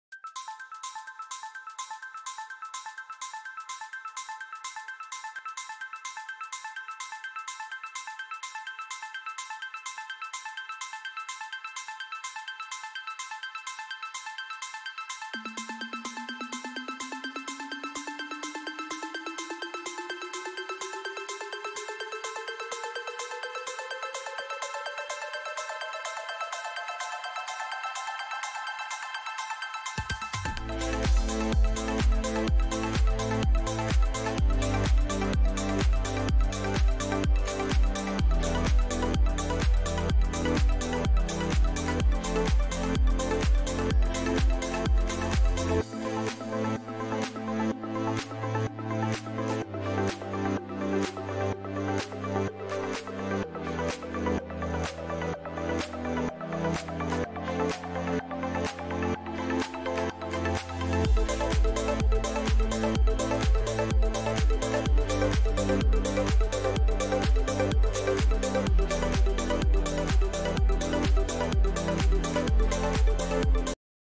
Day 43 PBB Updates GIN, ISABEL, AT ROQUE ISA KAYA SA KANILA ANG NOMINADO MAMAYA?
PBB Season 10 Adult Housemates Edition